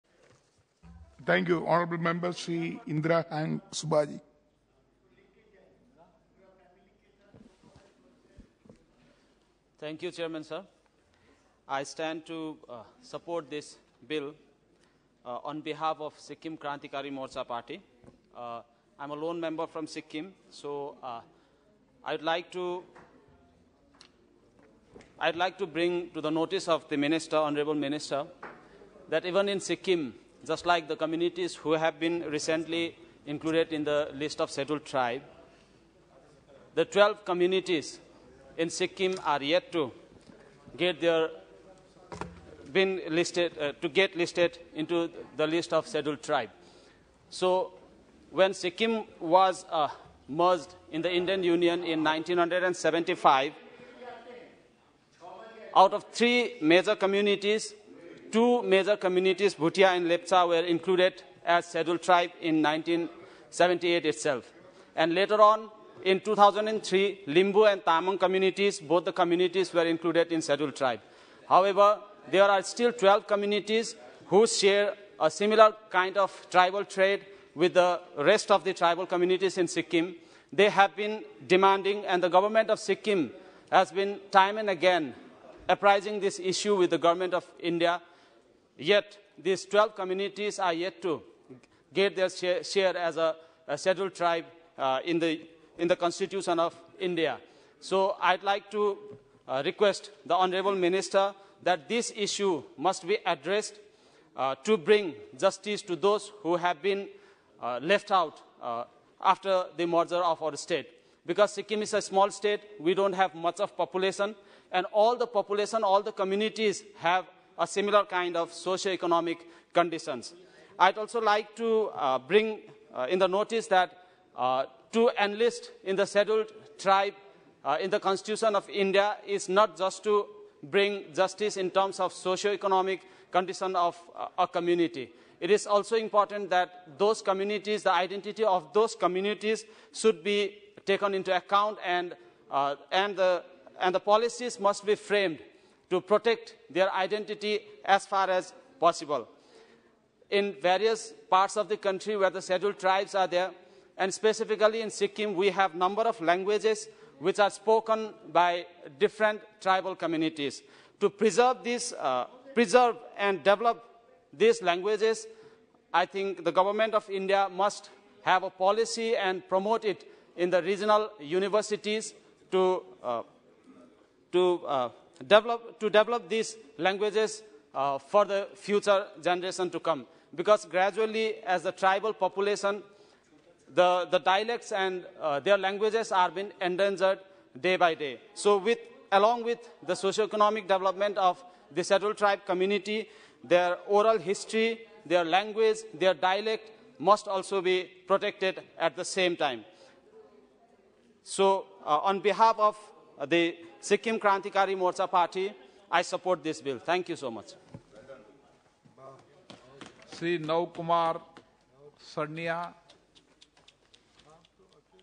Indra Hang Subba | The Constitution (Scheduled Tribes) Order (Third Amendment) Bill, 2022
Discussion on The Constitution (Scheduled Tribes) Order (Third Amendment) Bill, 2022 in Lok Sabha. The Bill further amends the Constitution (Scheduled Tribes) Order, 1950 to provide for inclusion of certain communities in the list of Scheduled Tribes in relation to the State of Himachal Pradesh.